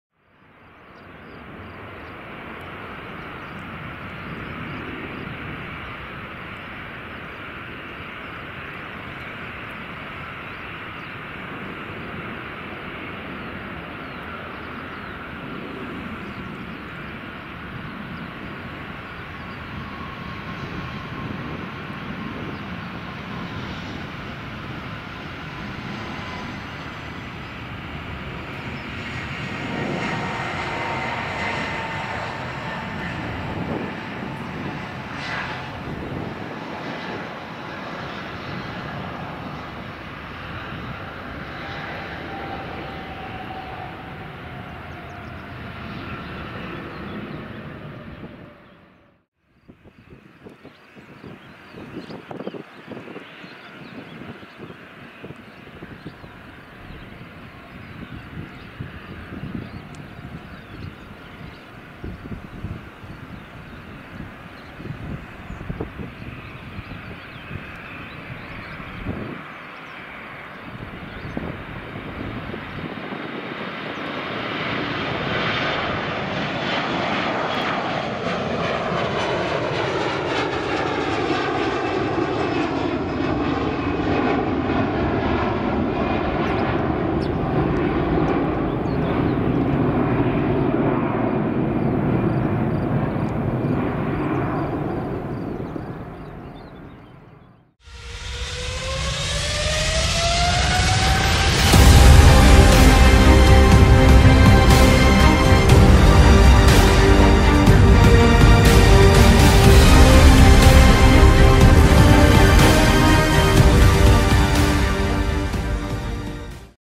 [FullHD] *NEW* Lumiwings B733 (SX-LWA) Taxi+Take off at Munich Airport!
One of the newest airlines in europe this is the only one aircraft from Lumiwings from greece doing some charterflights for Astra enjoy!
From: Munich
To: Thessaloniki
Airline: Lumiwings (Op.By Astra Airlines)
REG: SX-LWA
Aircraft: Boeing 737-300